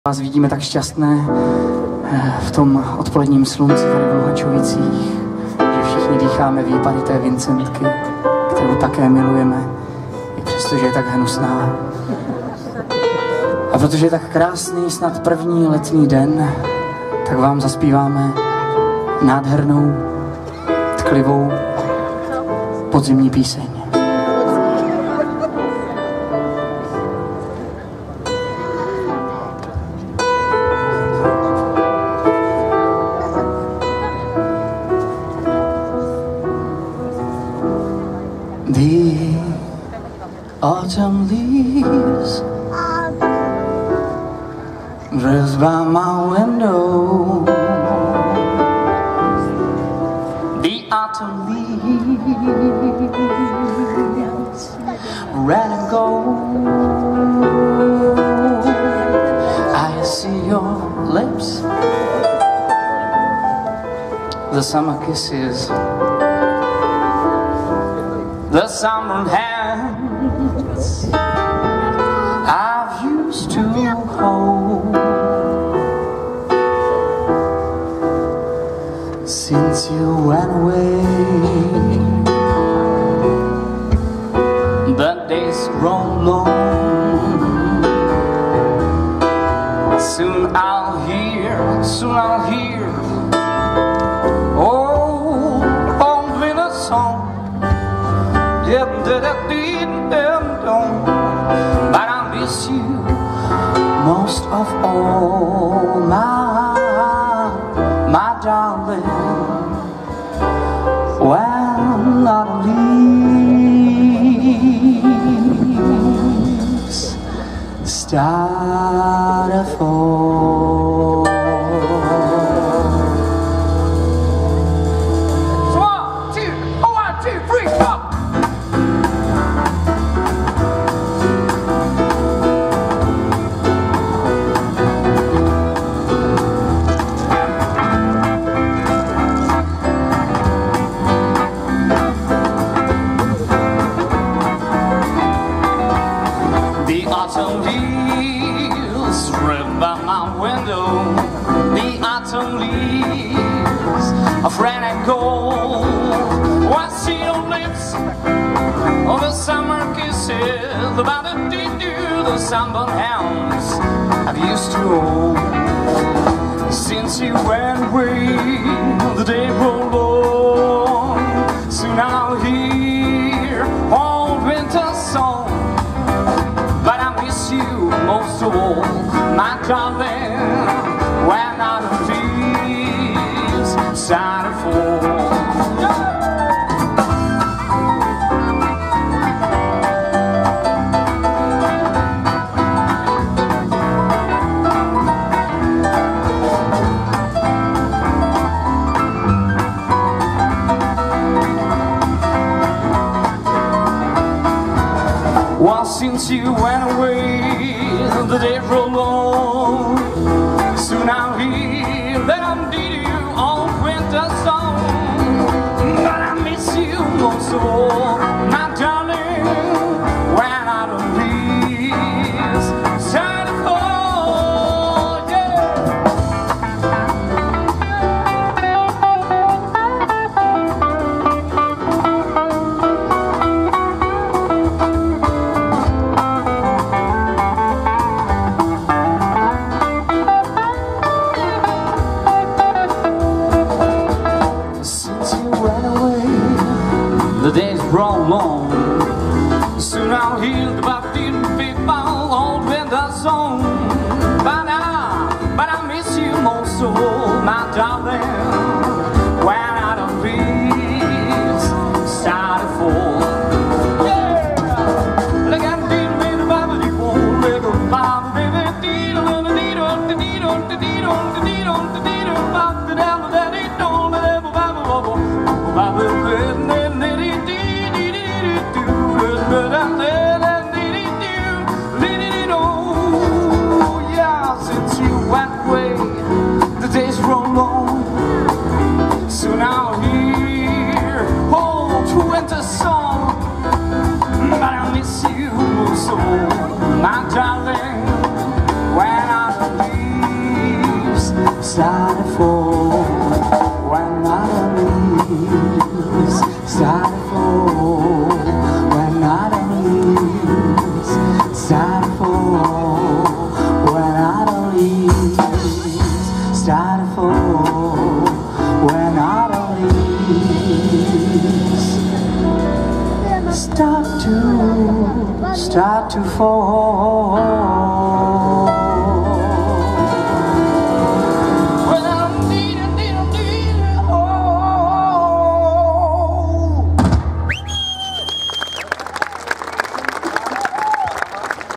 Skupina Golden Delicious - ploužák
Koncert skupiny Golden Delicious (Zlín) se konal při příležitosti otevření pramenů 2016 v lázeňském městě Luhačovice před společenským domem.